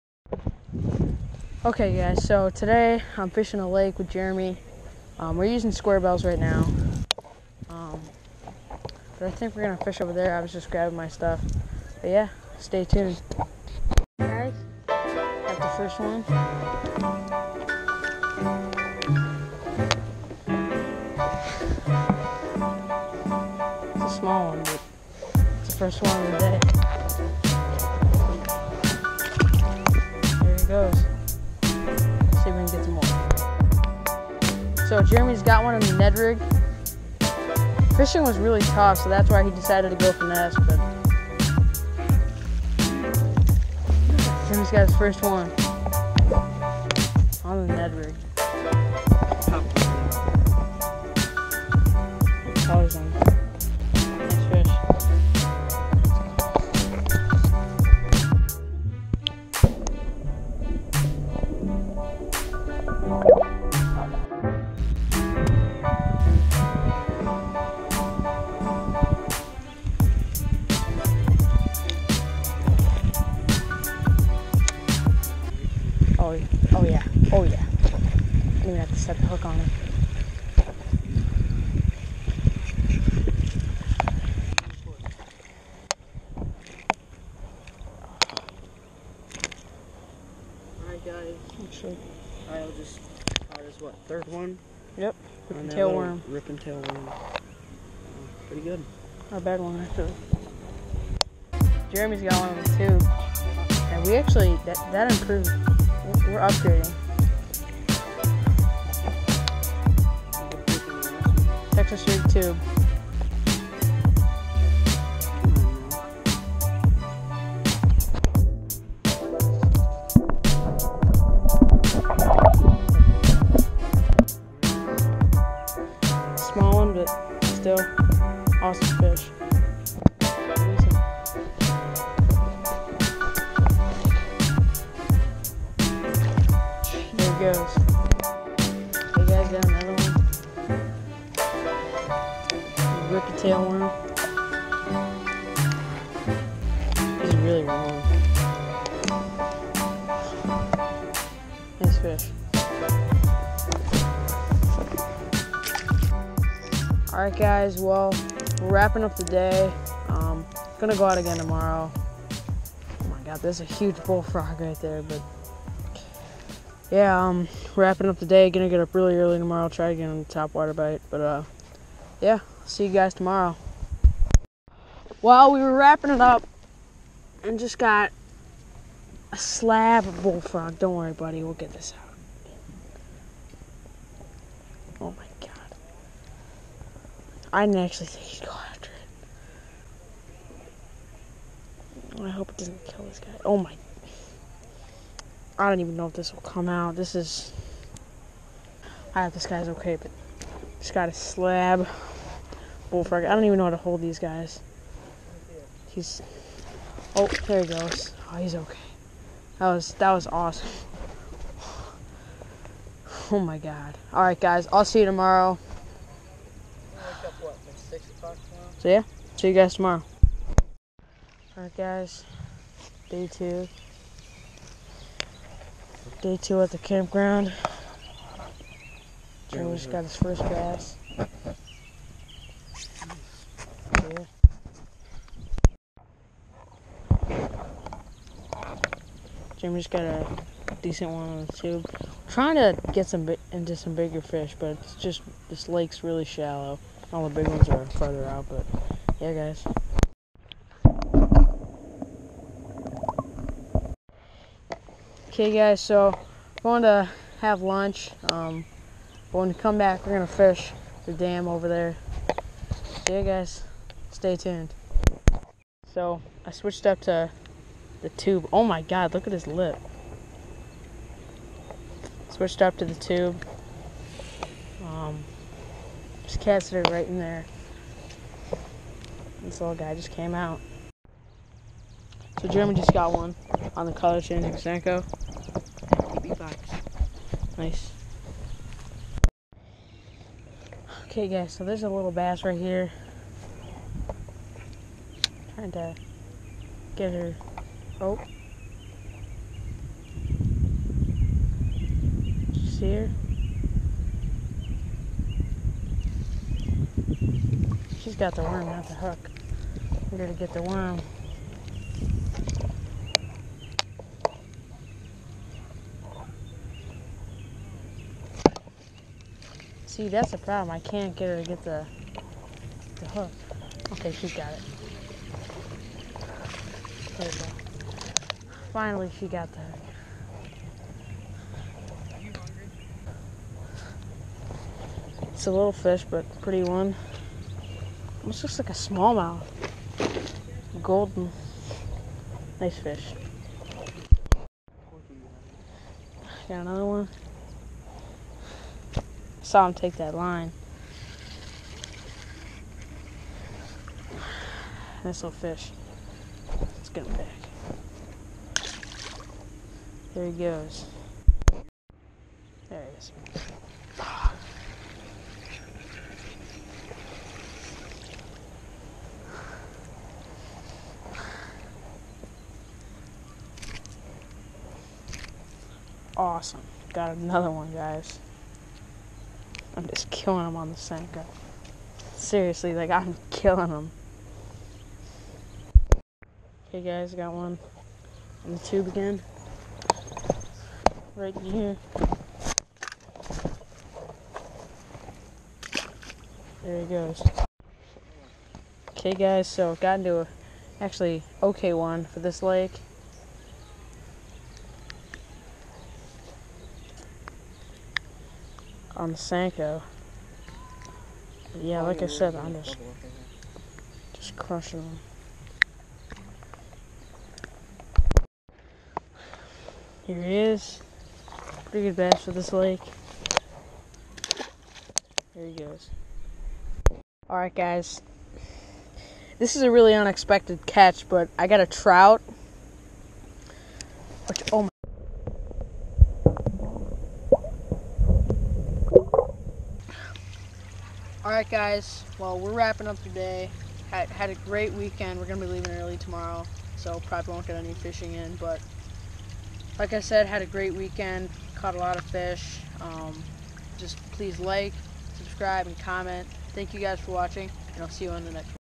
Fishing Rustic Lake
Me and Jeremy went to his campground to fish and caught a lot of fish. Enjoy!